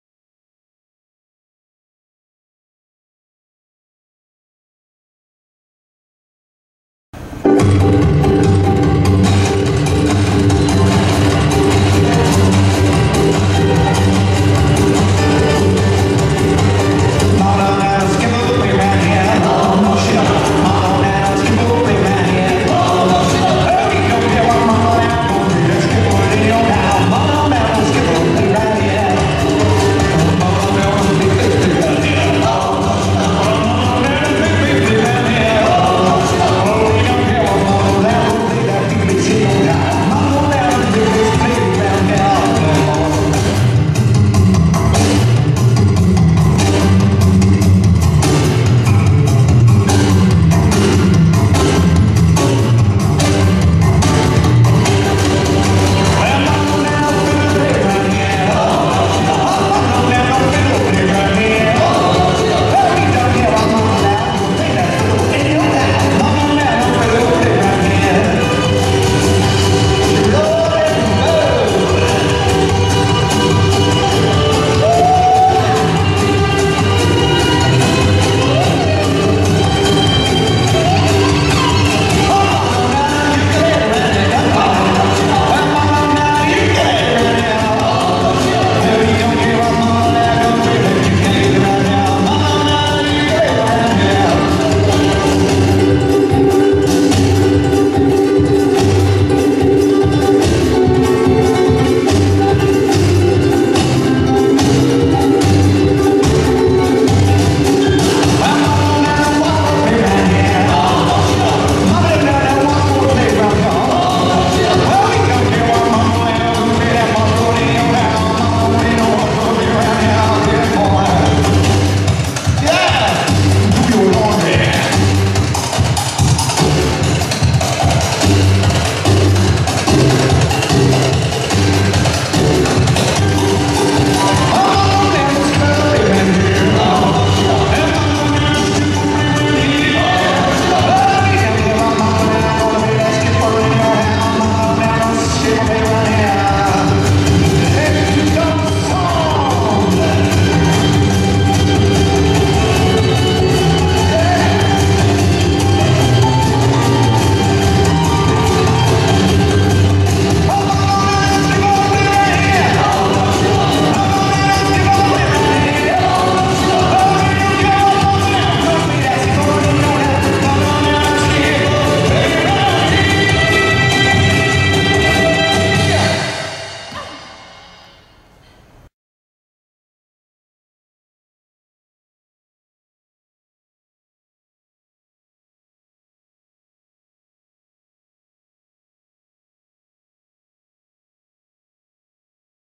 Skiffle Time (Demo by Silver Diamond Dancers)
Skiffle Time
64 Count - 4 Wall - Improver & Intermediate Level
Choreographed by Darren Bailey